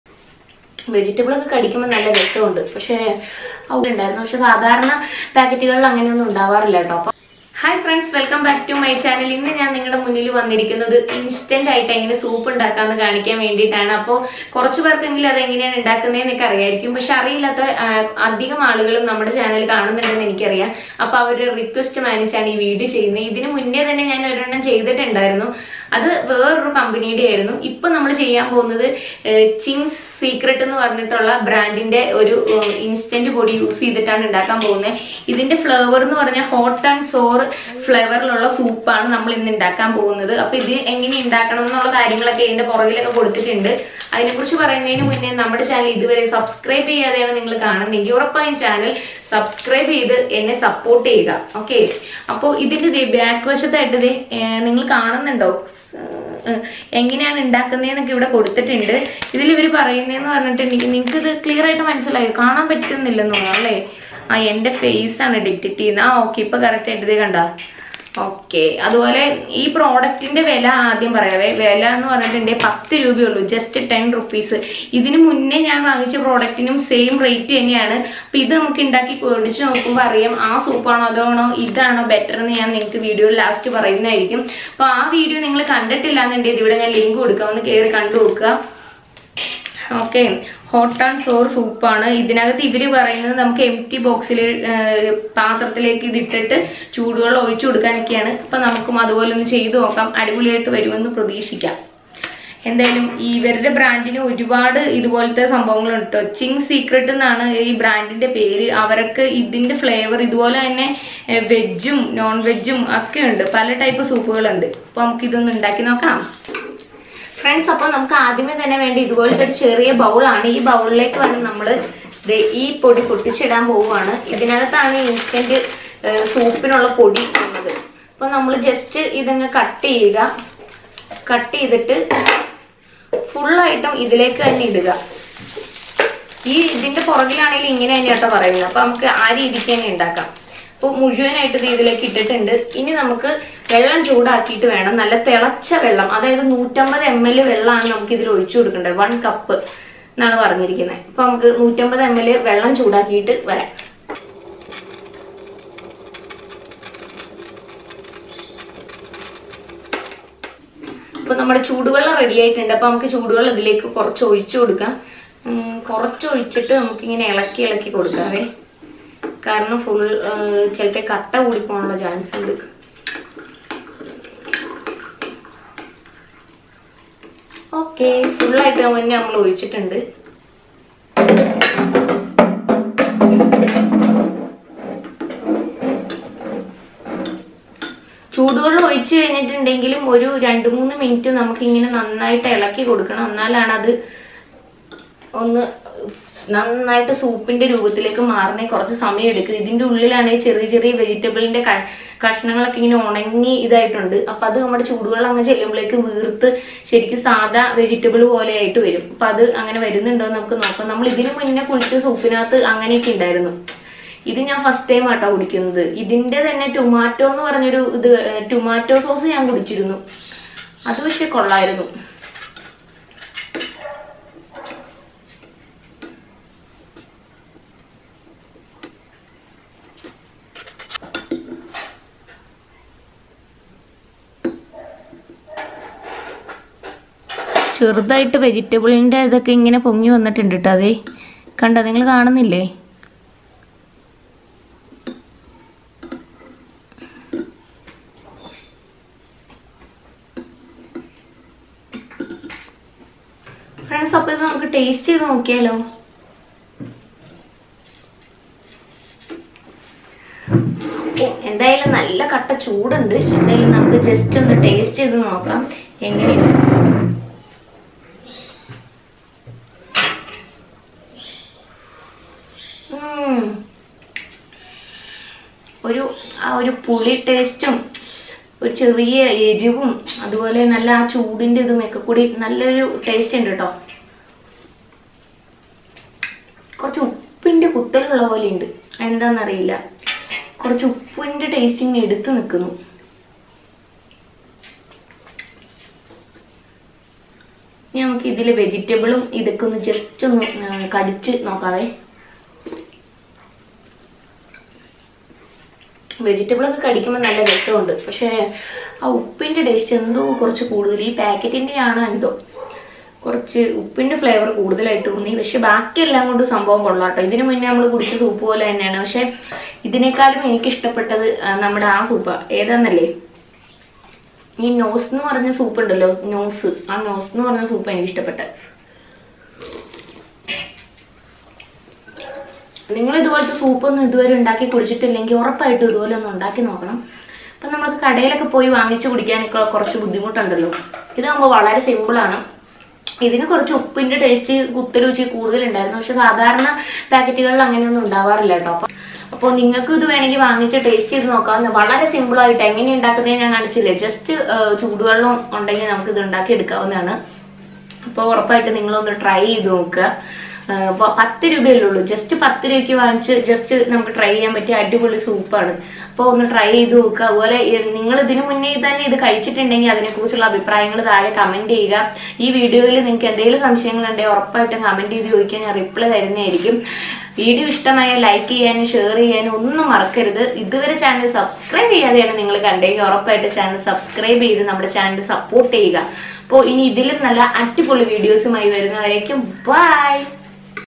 Ching's secret hot and sour soup | ching's secret hot and sour soup recipe
Ching's secret hot and sour soup | ching's secret hot and sour soup recipe | ching's secret soup | Saranya Boban Vlogs

Deliciously spicy hot and sour soup by Ching's is ready in a minute to satiate any-time hunger pangs instantly. Just take a cup of boiling water, mix the contents of the Ching's Secret Instant Hot & Sour Soup and wait for a minute before savouring a garam-a-garam cup of this desi-spiced Chinese soup.

Ching's Secret Hot & Sour instant soup is not only easy to cook but also fortified with real vegetables to ensure you get some nutrition without compromising on the taste. Enjoy this hot soup with a piece of bread for a light meal or sip on it, at any time of the day or night, to bust your hunger pangs without consuming any unnecessary calories.

instant soup